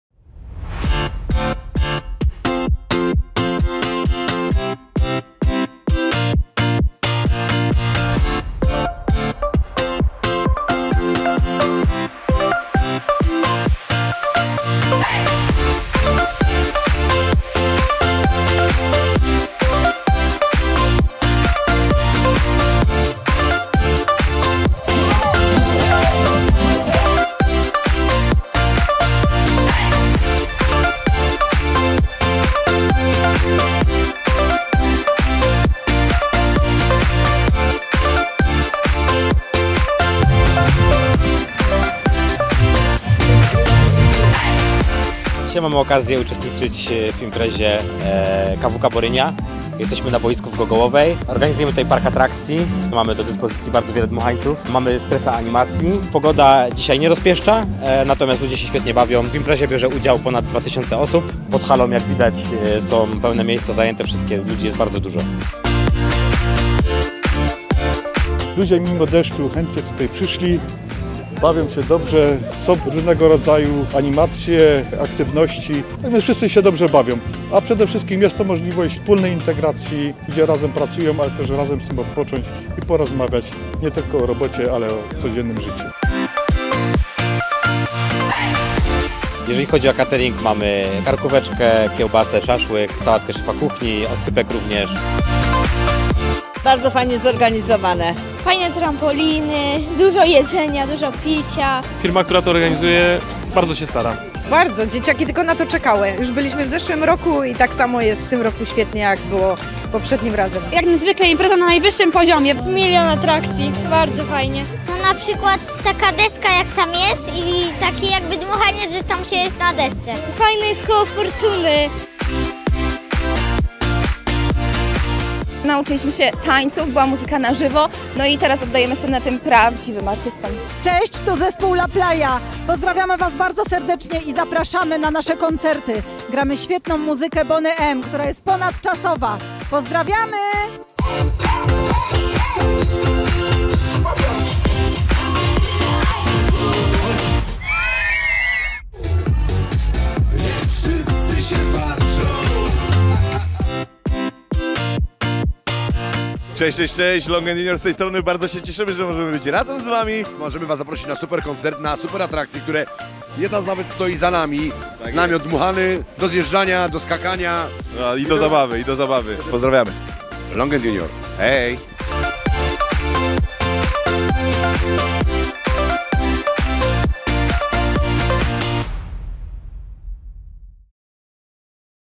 Piknik rodzinny Dzień Dziecka JSW KWK Borynia-Zofiówka-Bzie Ruch Borynia realizacja imprezy Dart-Pol
Kompleksowa organizacja imprezy, usługa gastronomiczna wydawana bez limitu, bogaty program artystyczny. Charakterystyka eventu : 2000 uczestników, brak kolejek do gastronomi i atrakcji.
Gastronomia wydawana bez limitu